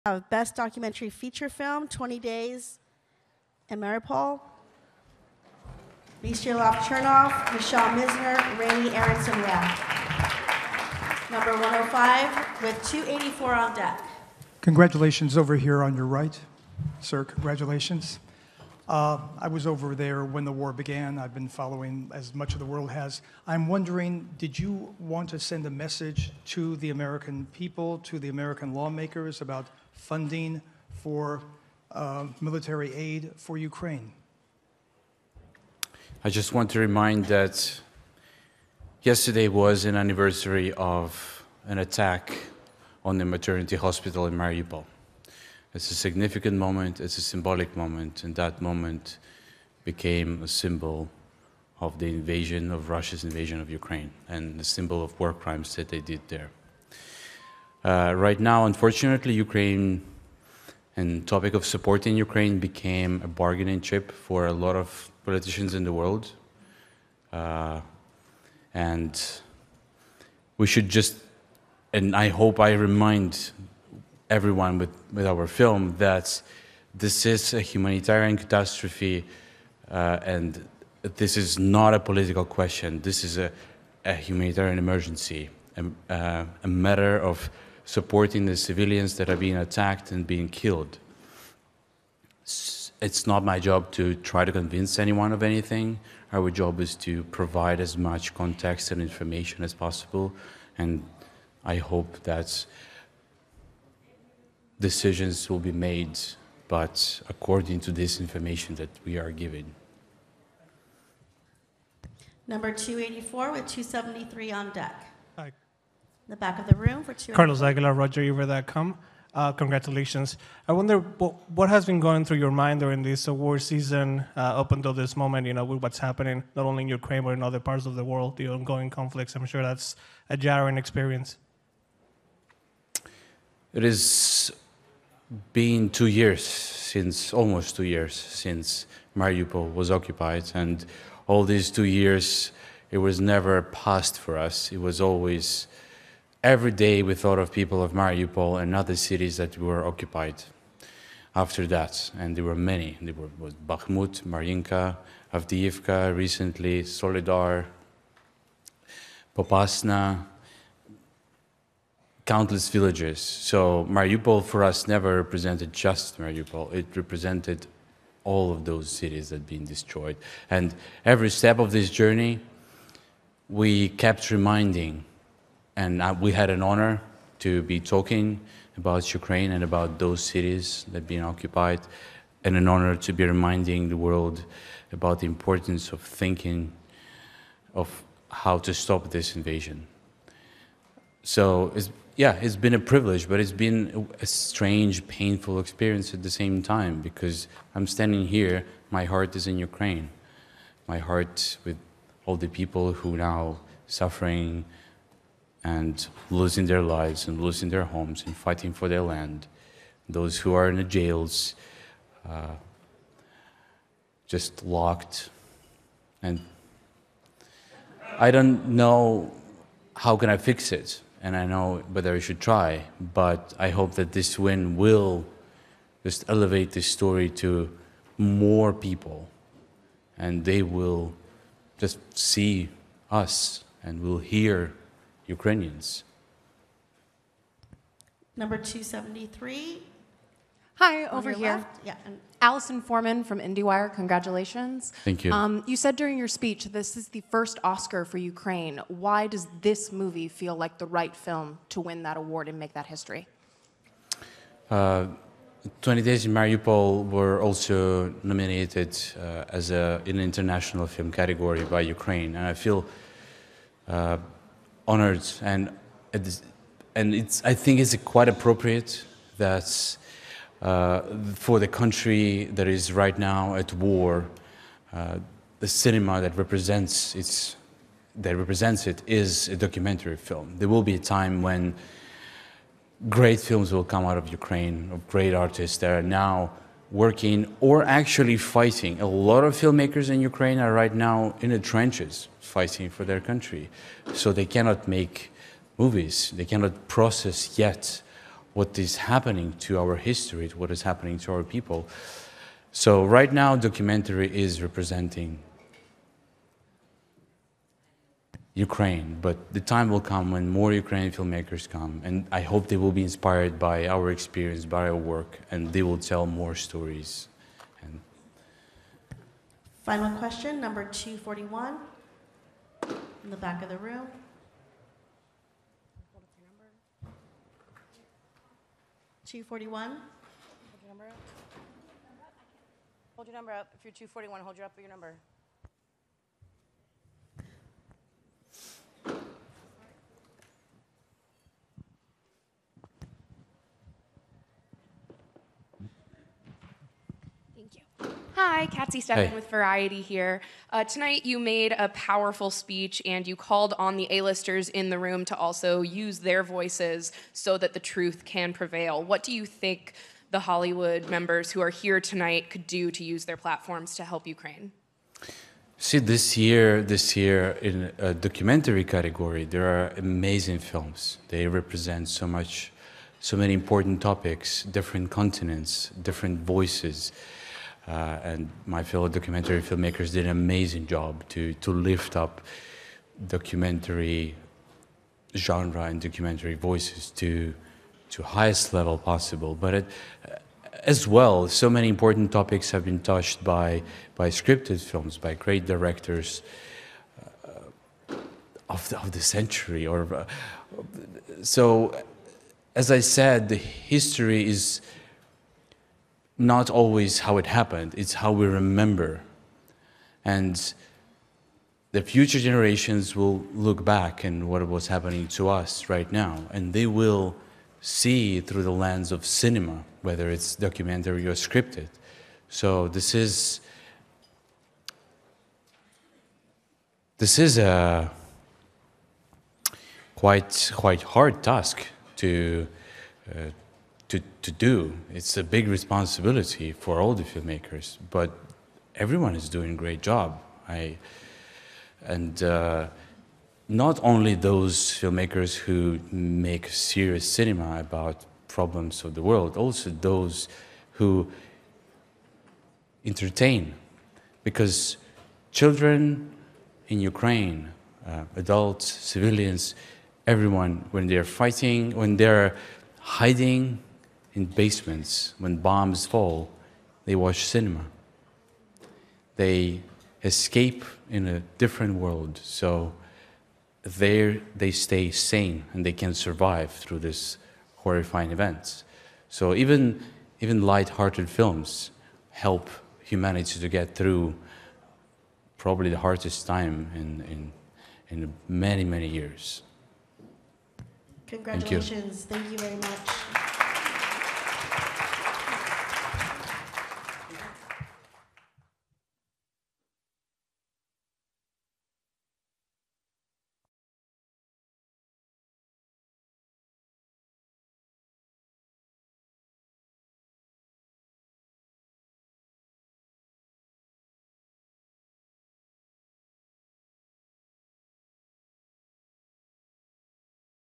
Best Documentary Feature Film | '20 Days in Mariupol' | Oscars 2024 Press Room Speech
Best Documentary Feature Film '20 Days in Mariupol' winners Mstyslav Chernov, Michelle Mizner and Raney Aronson-Rath speaking to acclaimed press moments after Oscar win.

The 96th Oscars was held on Sunday, March 10th at 7e/4p, and televised LIVE on ABC and in more than 200 territories worldwide.